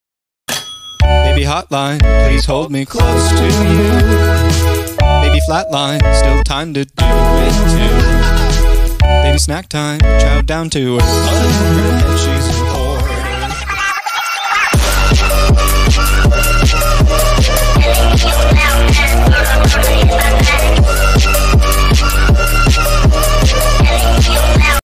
Can't stop the pain train
it's an animation I made with moon animator